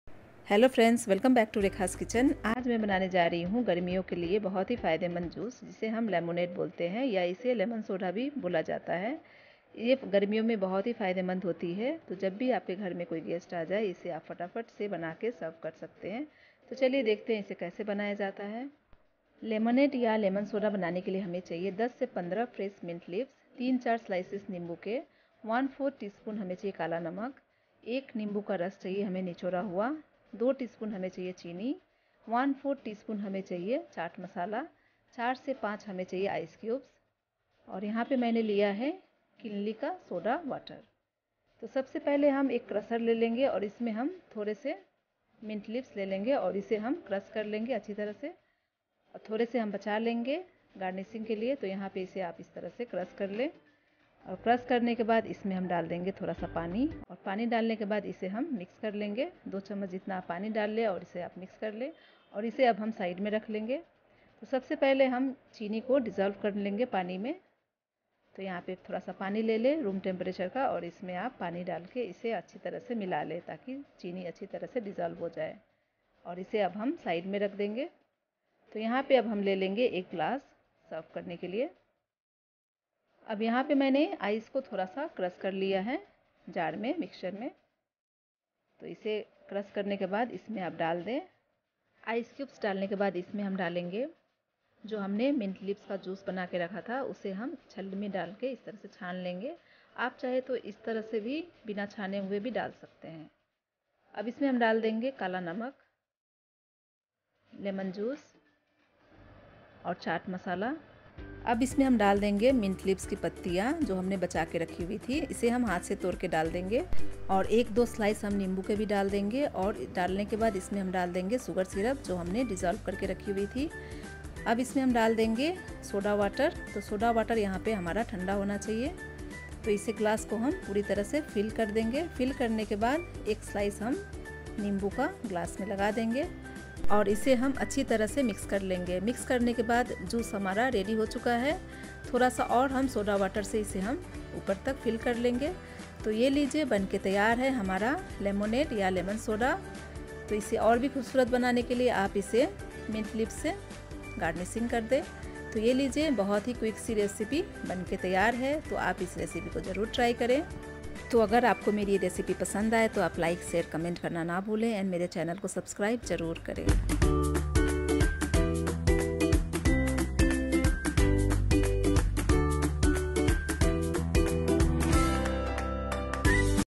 2 min Lemonade recipe | Restaurant Style Lemon Soda recipe | Summer Special | Easy and Quick
Presenting before you how to make Easy 2 minute Lemonade Recipe.
See y’all in my next video with a new recipe! Until then, bye!